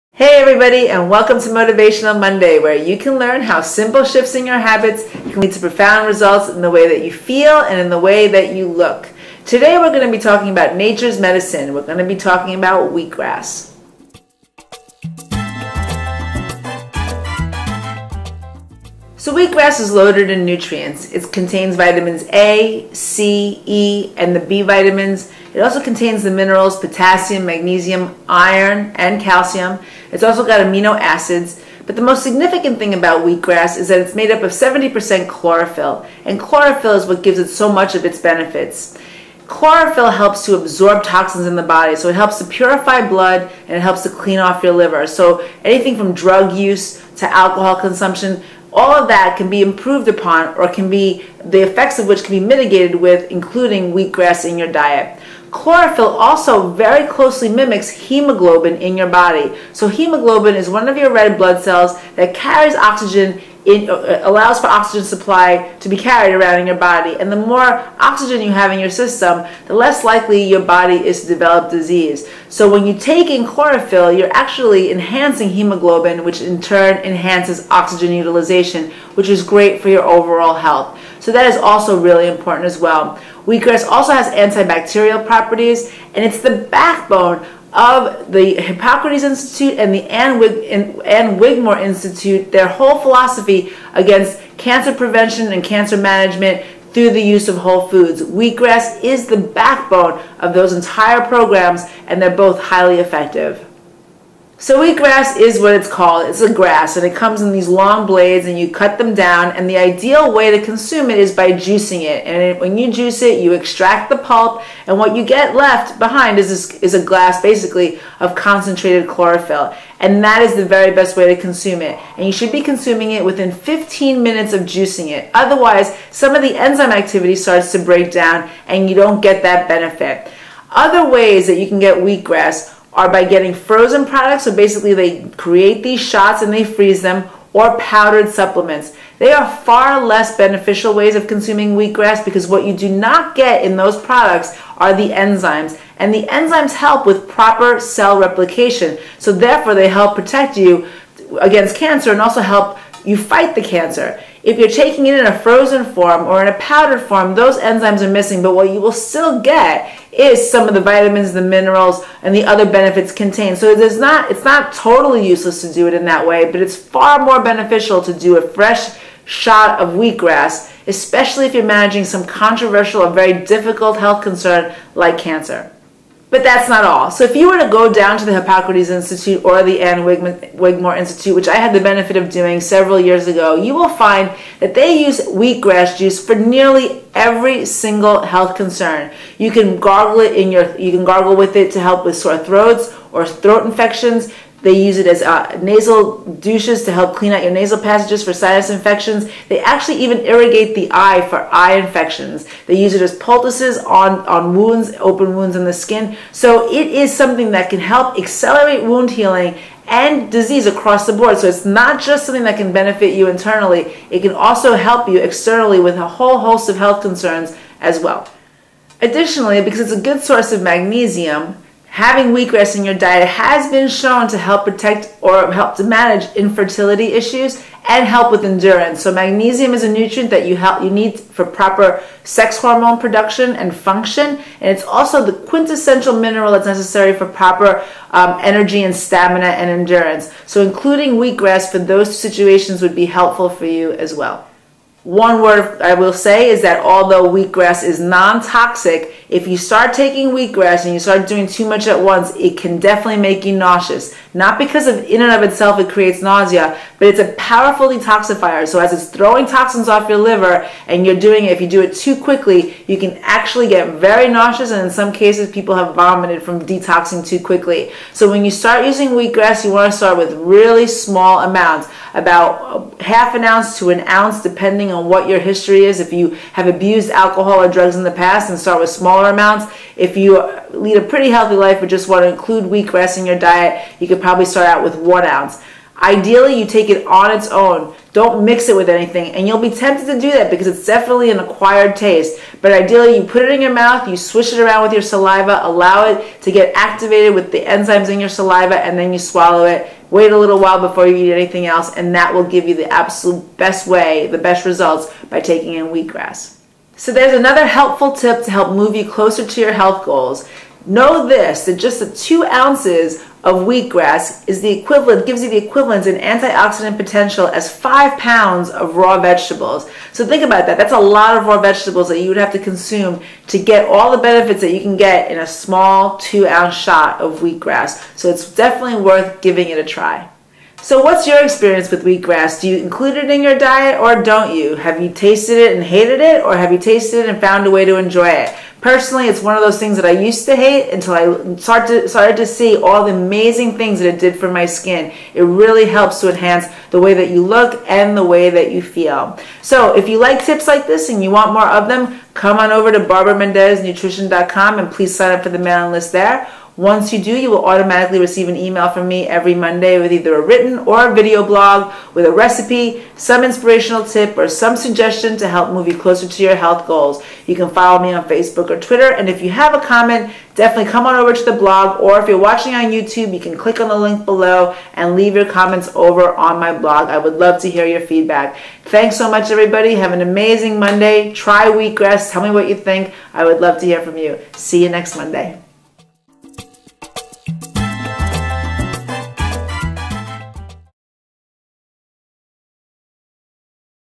The Benfits of Wheatgrass
Hey Everyone! I hope you all enjoyed a happy Thanksgiving!

Thanksgiving weekend has always felt like one big rolling party for me. There's the day itself and then come Friday, there’s my camp reunion party hosted by my friend M-J that has been happening for over 30 years now! This year was particularly special because my cousin got engaged, so the celebrations were continuous. It was a spectacular weekend full of too much food and wine and today I am ready to detox!

I don’t usually recommend a full-on detox in the middle of winter though. Detoxes are cold in nature because of all the juices and lack of solid food so to try and adhere to something like that when it’s so cold out can make it hard to have energy, focus and concentration. What I DO recommend though is cleaning up the diet, adhering to lots of vegetables, fruit and some protein. I also suggest drinking lots of water and then including wheatgrass shots to help facilitate the detoxification process.

If you’re not familiar with Wheatgrass and its benefits then you’ll want to catch today’s video. Loaded in vitamins, minerals, enzymes and the health enhancing benefits of chlorophyll, Wheatgrass is nature’s medicine. You can use it to boost immune function, help fight anemias, enhance stamina and energy, improve the quality of your skin and even help with fertility. It’s one of the very best things you can do to improve the quality of your health.

Once you’ve watched the video, I’d love to hear from you! Do you have experience with taking Wheatgrass shots? Have you improved upon or cured a medical condition because you included wheatgrass in your diet? Let us know in the space below.

Wishing you all a happy, healthy week! I’m off to do my wheatgrass shot~
xo
B